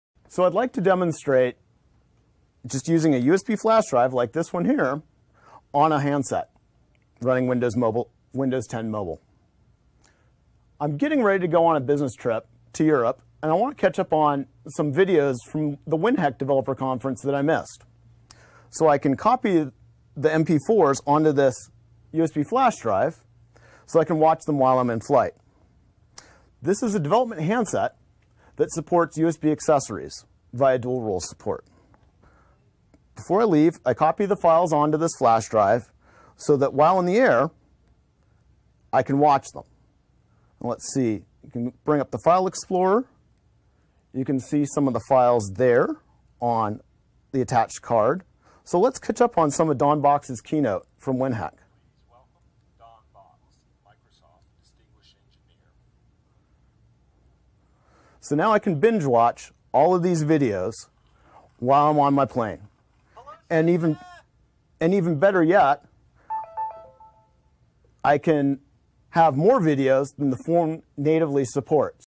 Windows 10 Mobile : USB OTG Support
Demo of USB OTG Support on Windows 10 Mobile by Microsoft at Build 2015